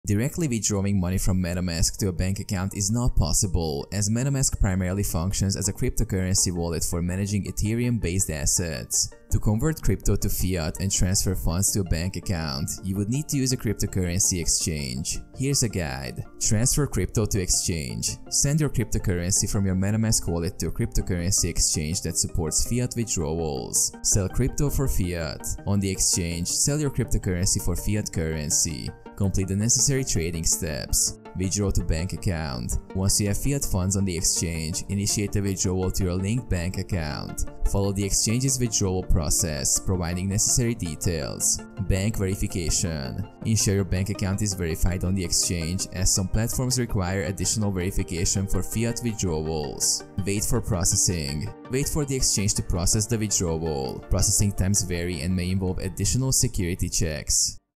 How To Withdraw Money From Metamask To Bank Account?
In this video I will explain How You Can Withdraw Money From Metamask To Bank Account.